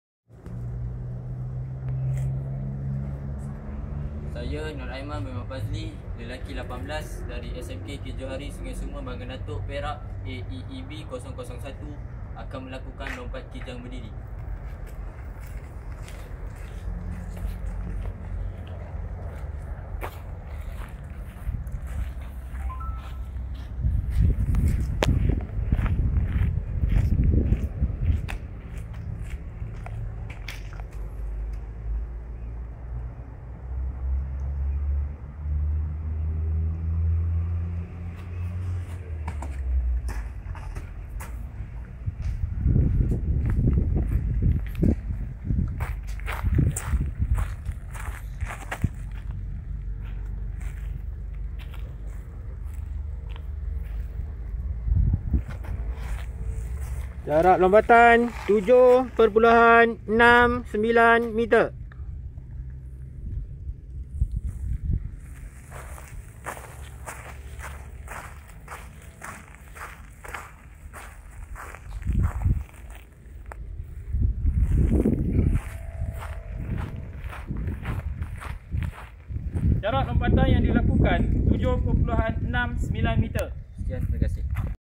MSS PERAK OLAHRAGA LOMPAT KIJANG BERDIRI L18 LELAKI P1
NAMA : NOOR AIMAN BIN MOHD PAZLI
SEKOLAH : SMK KHIR JOHARI
DAERAH : BAGAN DATUK
KOD SEKOLAH : AEEB001